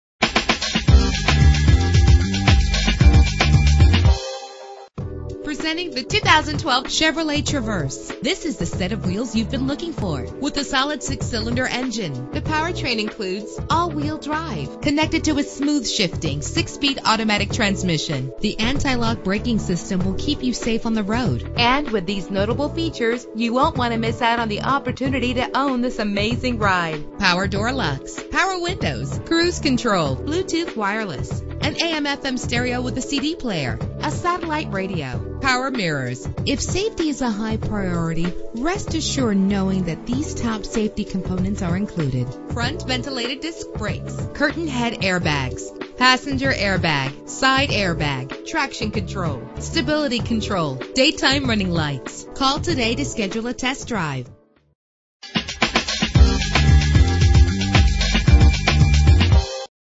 2012 Chevrolet Traverse - Ephrata Pa
Year: 2012
Make: Chevrolet
Model: Traverse
Trim: 1LT
Engine: 3.6 liter 6 cylinder 24 valve
Transmission: 6-Speed Automatic
Color: White
Mileage: 23558
Address:
1928 West Main Street